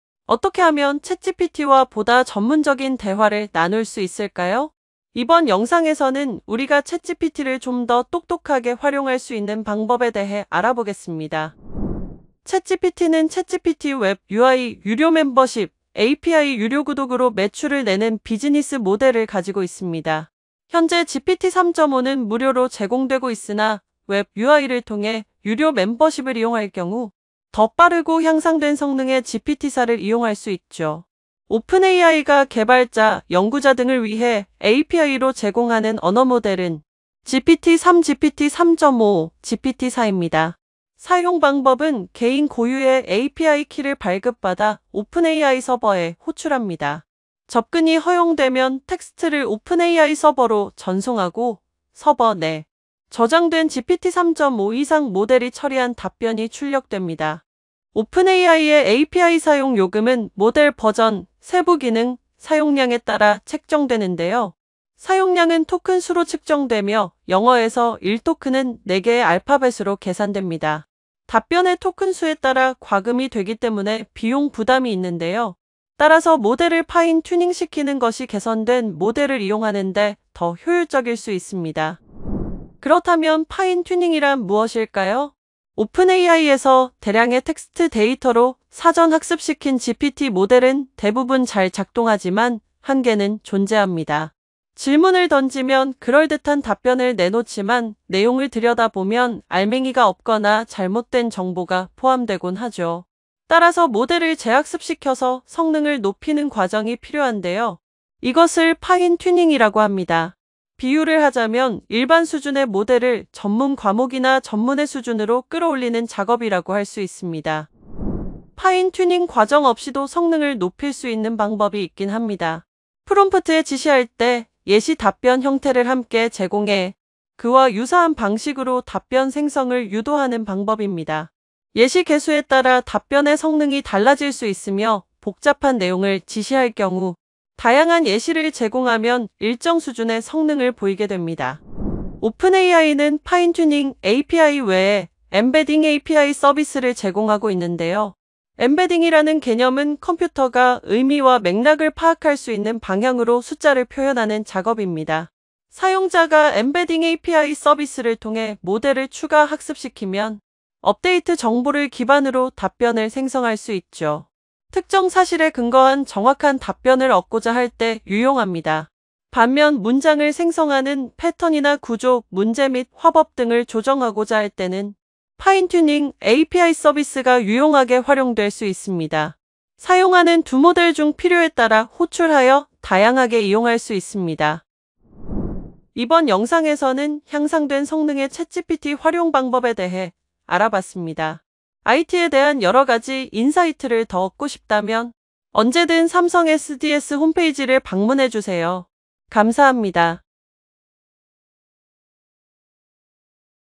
[3분 IT 인사이트] 효율적인 챗GPT 사용법 3분만에 배우기 💡 | ChatGPT 잘 쓰는 법, 챗GPT 4 활용, 생성형 인공지능 활용법, 챗지피티 사용법
챗GPT 기술 분석과 생성형 AI 시장 동향을 다루는 삼성SDS 인사이트 리포트.
이번 영상에서는 챗GPT를 좀 더 똑똑하게! 효율적으로! 활용할 수 있는 방법을 알아 봅니다.

본 영상은 생성형 AI 플루닛 스튜디오로 제작한 '인공지능 강사'가 등장하여
ChatGPT Web UI와 API의 활용, 더 나아가 ChatGPT의 전문성을 높이는 Fine-tuning(파인튜닝)을 소개합니다.

📌 챗GPT와 생성형 AI에 대한 더 자세한 내용은 삼성SDS 인사이트 리포트 'ChatGPT 기술 분석 백서' 를 통해 확인하실 수 있습니다.

📌 IT 기술 트렌드와 혁신 방향에 관한 인사이트💡가 필요하시면, 삼성SDS 유튜브 채널을 구독해 보세요👇

🤗 더욱 다양한 채널에서 삼성SDS를 만나보세요! 🤗